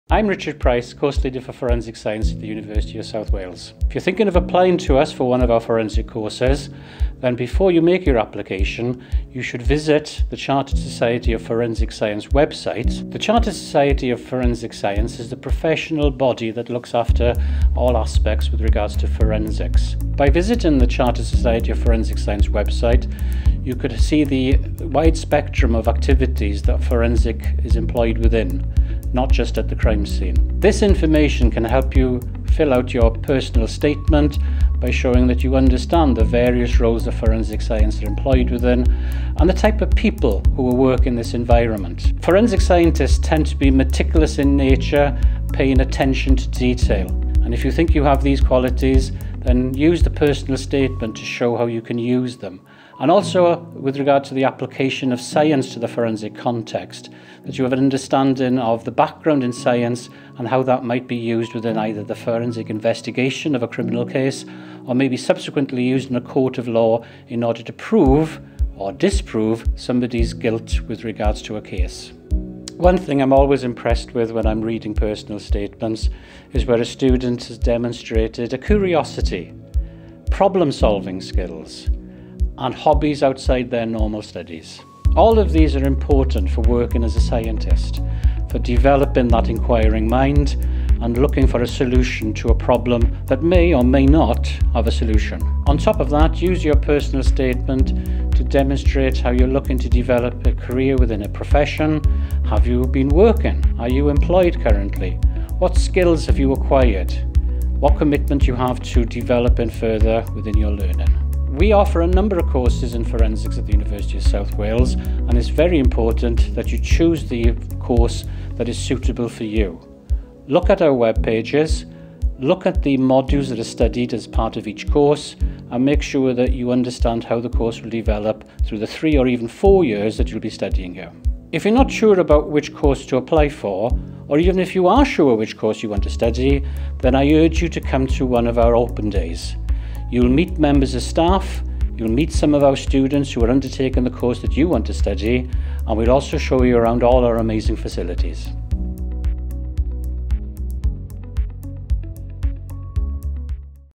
Forensic Science Degree | University Personal Statement Tips
If you are applying for a Forensic Science degree at Uni South Wales, course leader Richard Price has great advice for your personal statement.